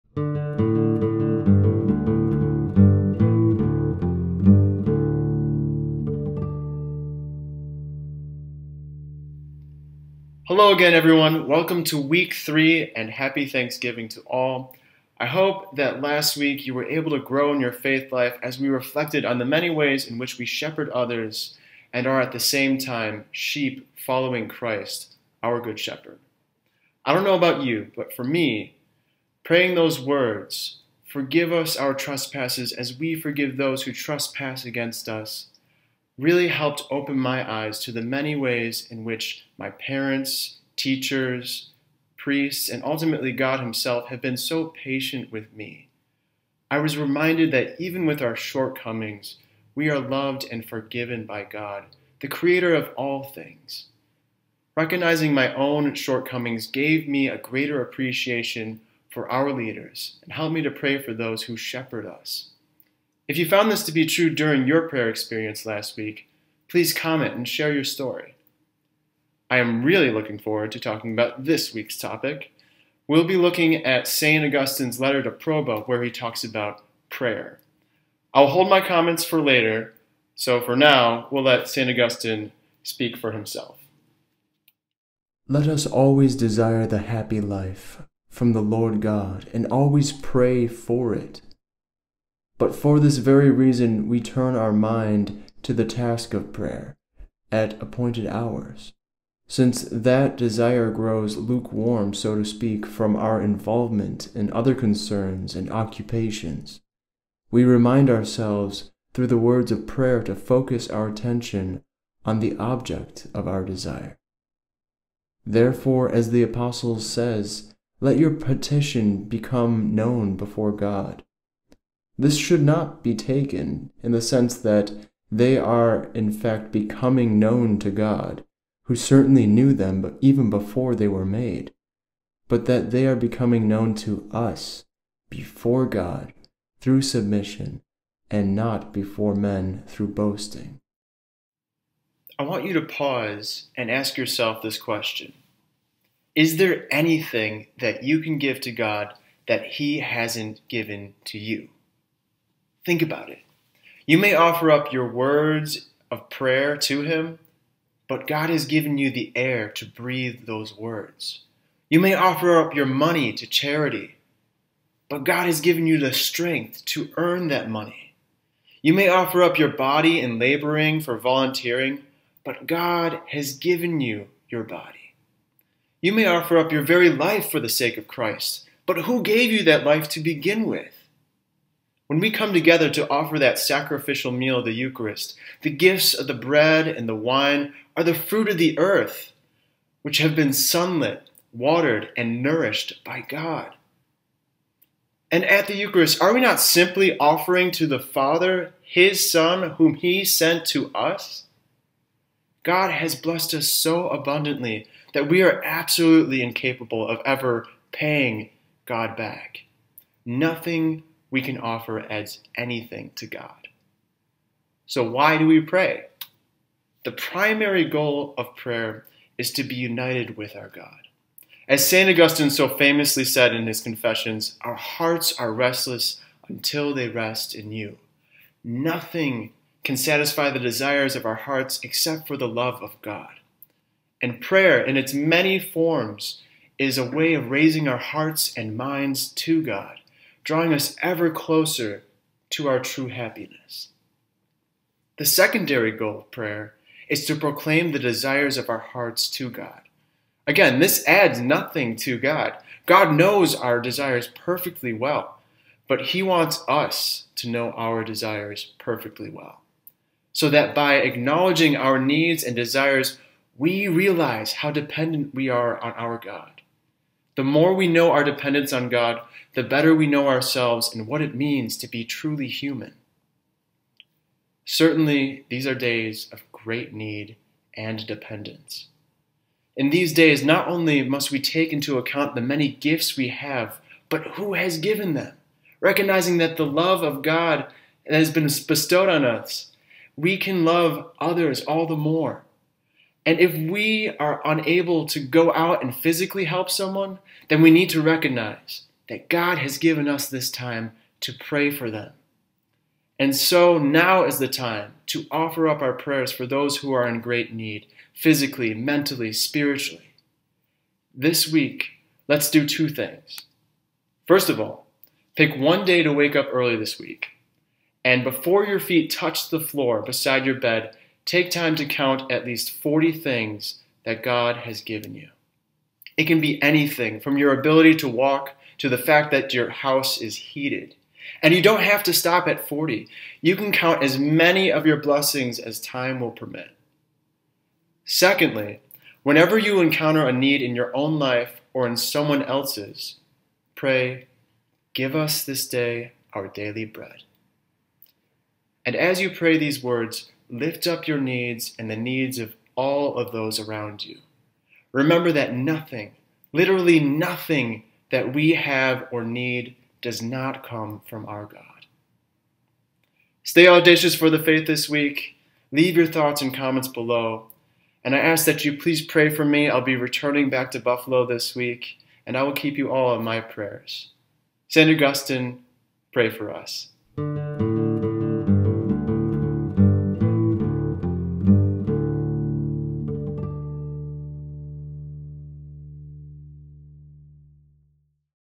Audacious Augustine 3
Brother James Bobak talks about St Augustine's amazing insights into prayer.